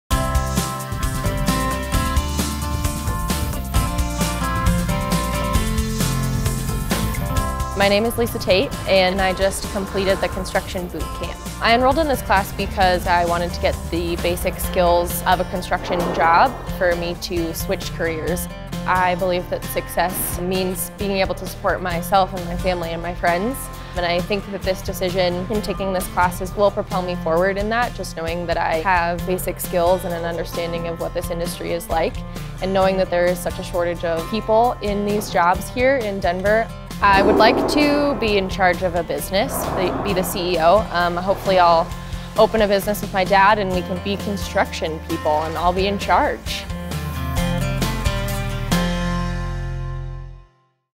"Become the CEO" - Lisa Tate, Bootcamp Grad
Lisa Tate is in the process of switching careers. The hands-on skills and industry knowledge that she gained in Construction Skills Bootcamp has set her on a path of becoming a construction business owner.